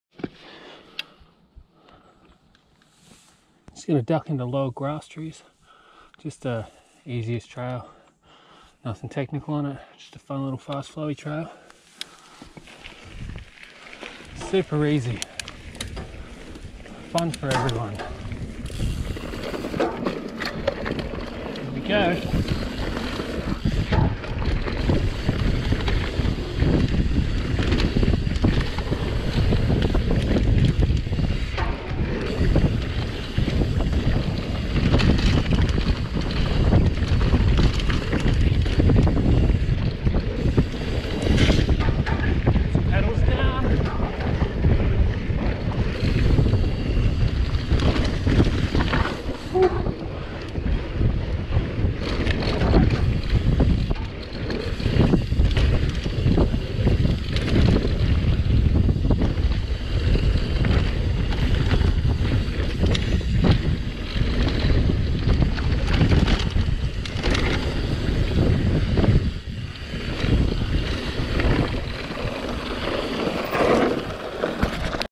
When Green Trails can still be fun - Grass Trees, Daisy Hill
A short run down Grasstrees East at Daisy Hill in Queensland, Australia. Its a green trail that is super easy but flowy and a fast and fun trail that everyone can ride. This run is on my Giant Reign 160mm travel Endruro rig and proves that even XC style trails can be ridden fast and fun on a long travel  mountain bike.